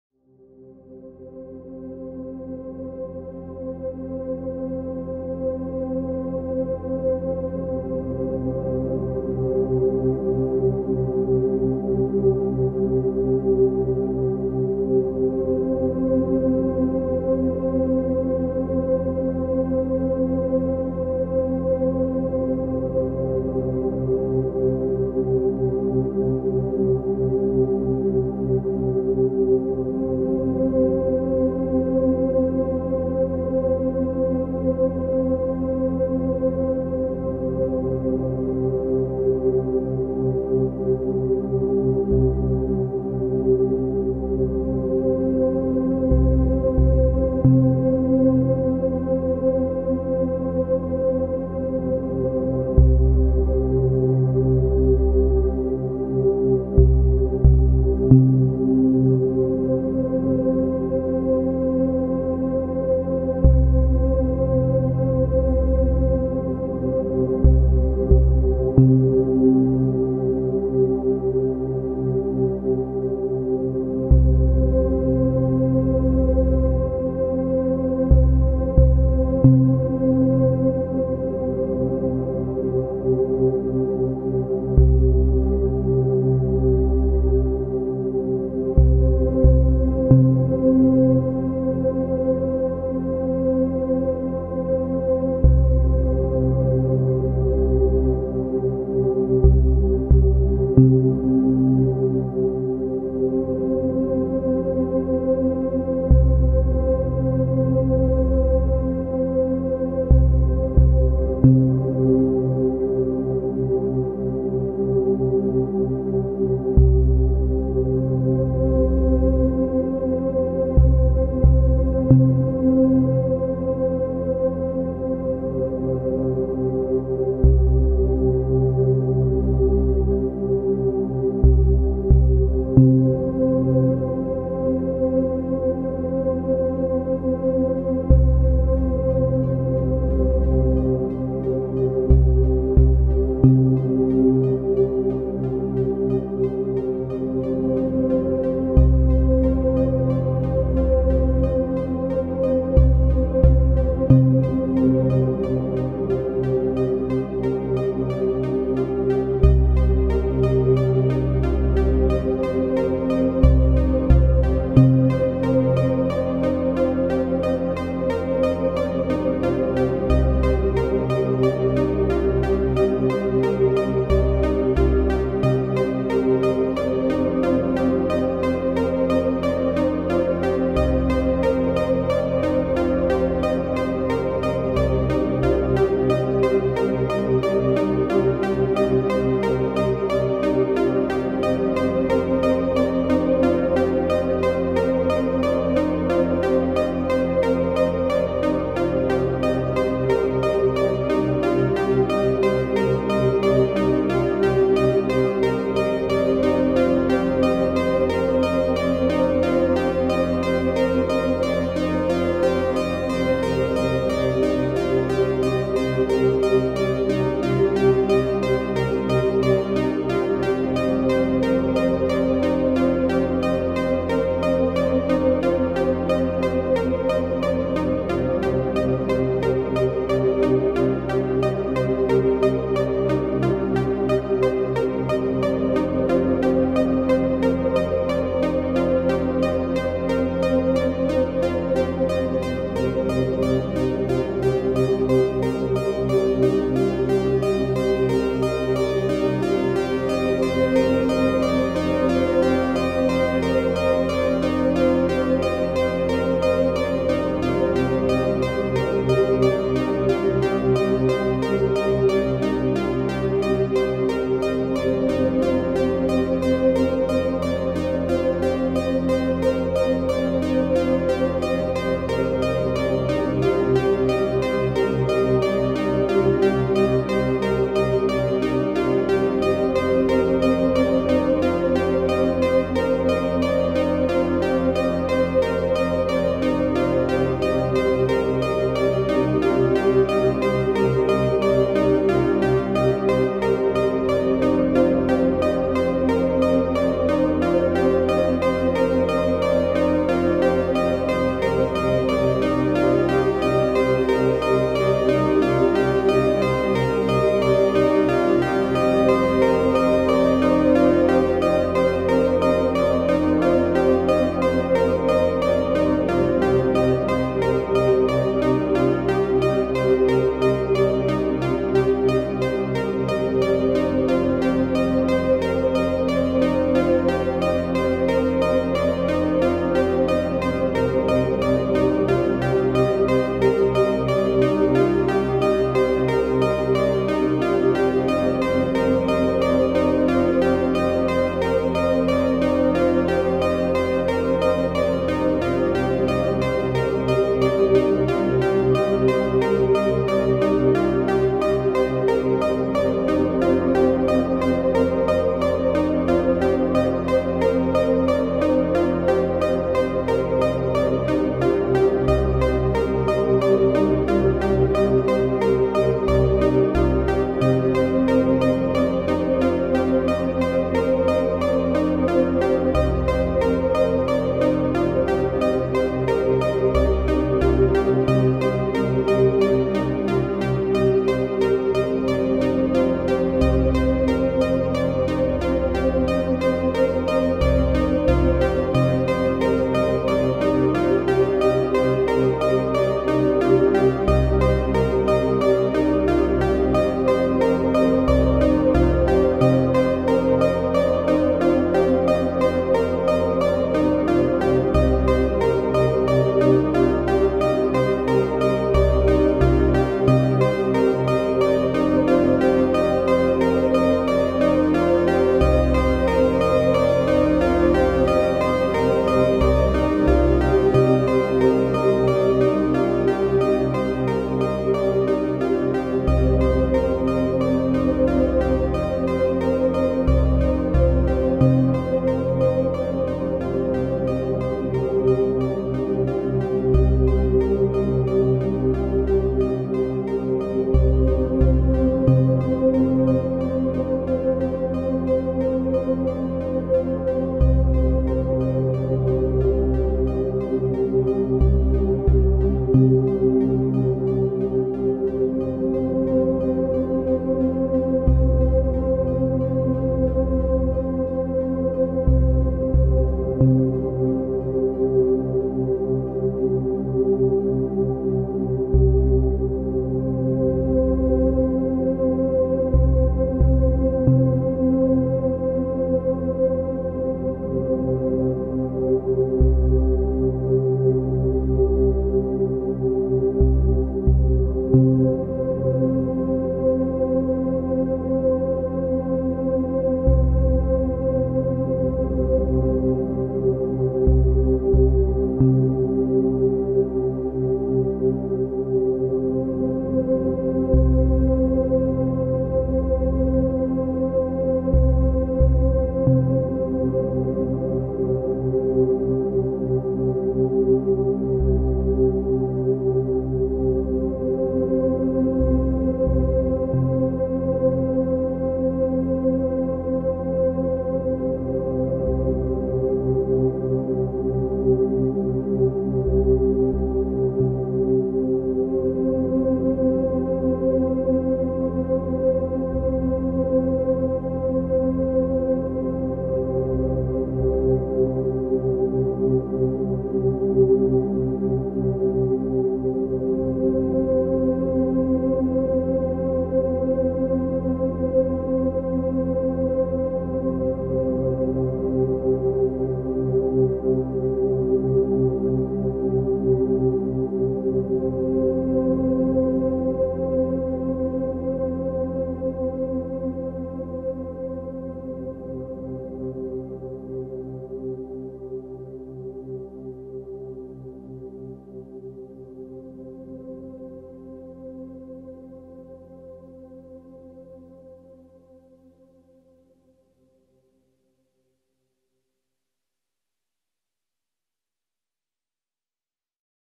Space Dream Ambient Music - Down Tempo - Beringer Crave - Volca FM
A new Project with Volca FM and Behringer Crave.
I have use Abliton Live to make the Atmosphere with Arturia Keyboard
Behringer Crave
Behringer TB3
Korg Volca Beats
Korg Volca FM
Arturia Keyboard